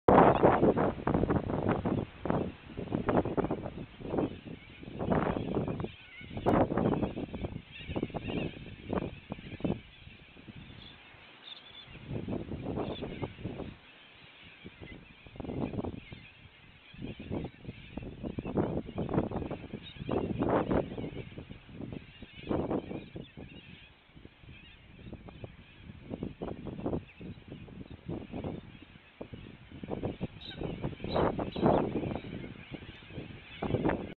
Quorn Corellas
Quorn, South Australia, on a windy day 100's of corellas rallied for a feed in the paddock, amazing to see how many were actually 'flying backwards' due to the strong wind!